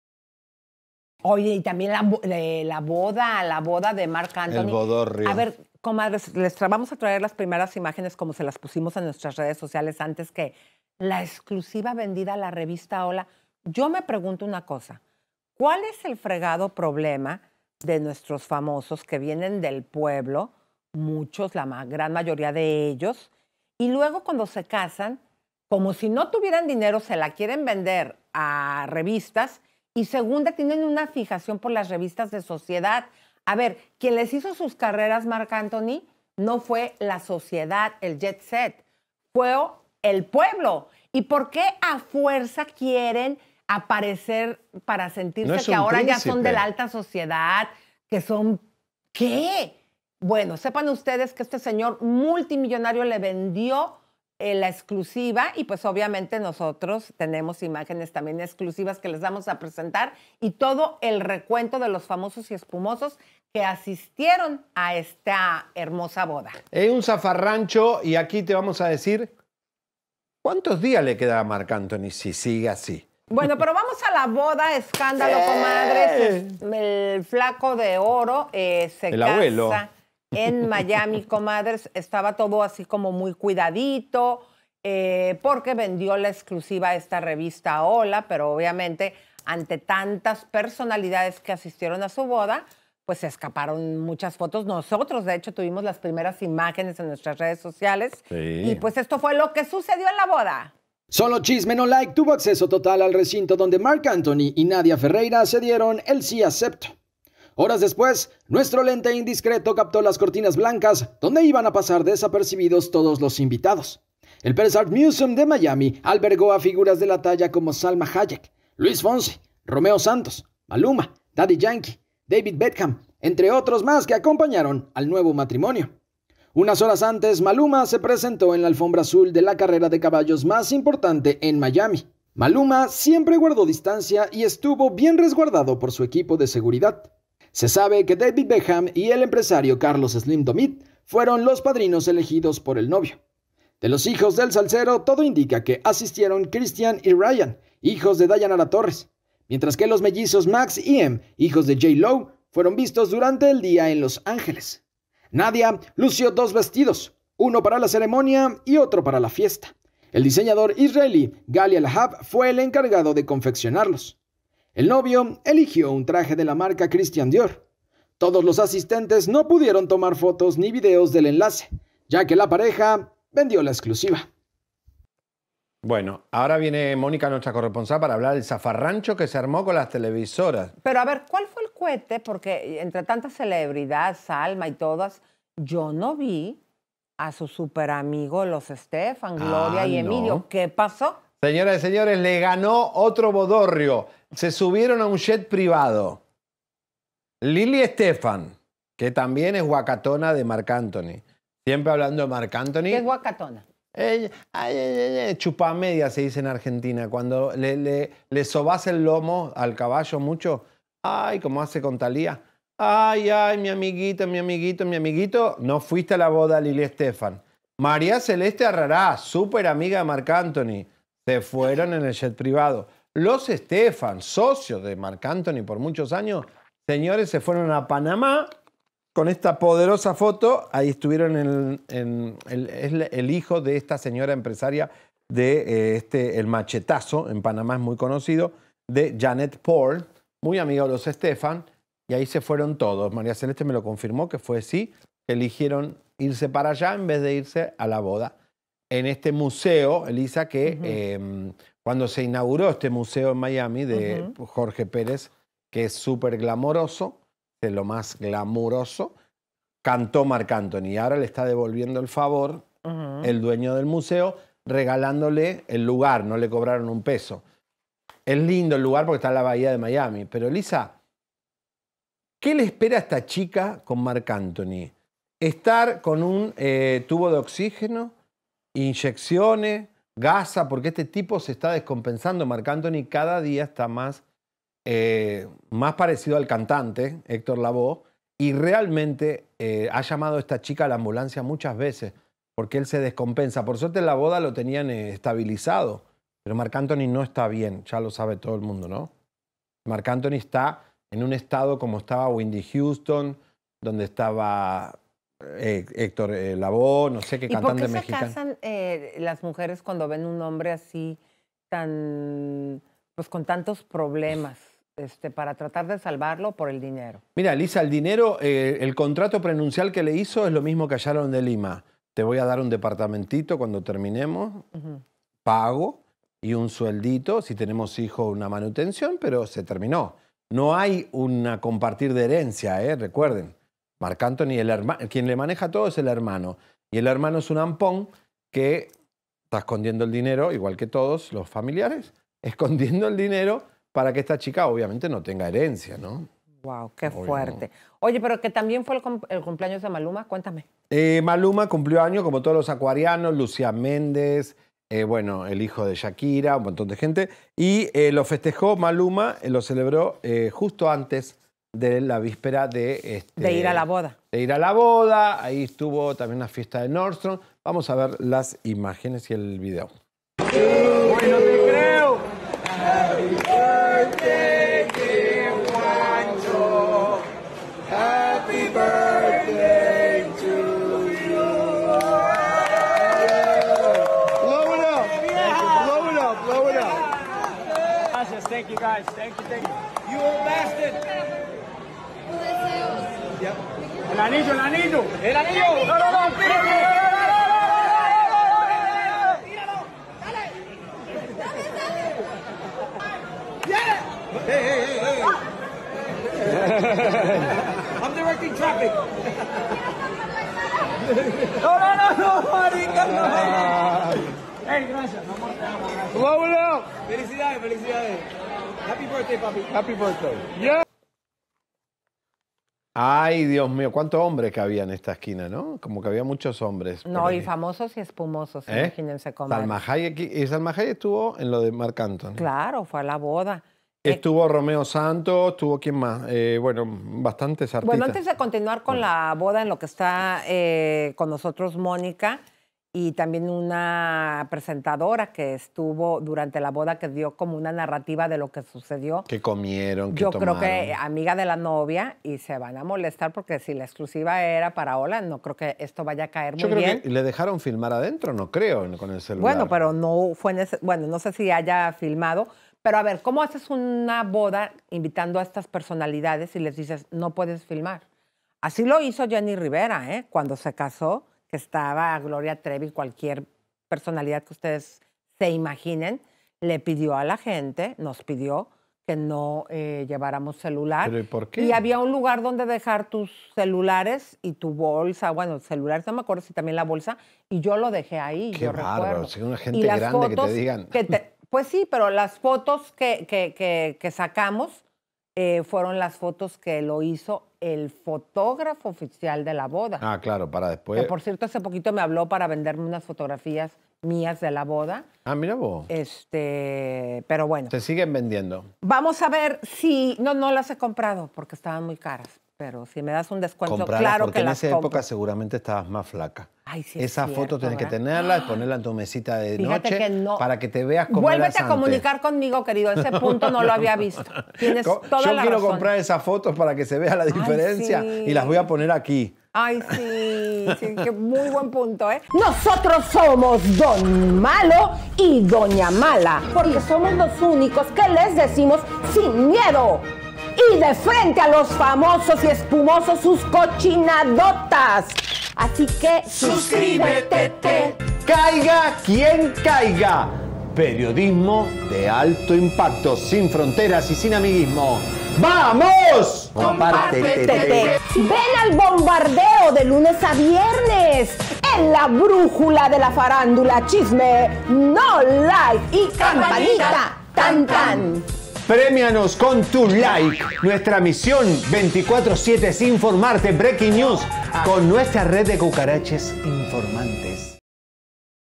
MARC ANTHONY SE EMOCIONA HASTA LAS LÁGRIMAS EN SU CUARTA BODA - Chisme No Like
ESTE FIN DE SEMANA SE DIERON EL "SÍ", MARC ANTHONY Y LA MISS UNIVERSO PARAGUAYA NADIA FERREIRA... EN UNA PRIVADISIMA Y HERMÉTICA CELEBRACIÓN A LA QUE LOS MEDIOS DE COMUNICACIÓN NO TUVIERON ACCESO… PERO PARA CHISME NO LIKE NO HAY IMPOSIBLES Y AQUÍ LO TENEMOS PARA USTEDES... 🤗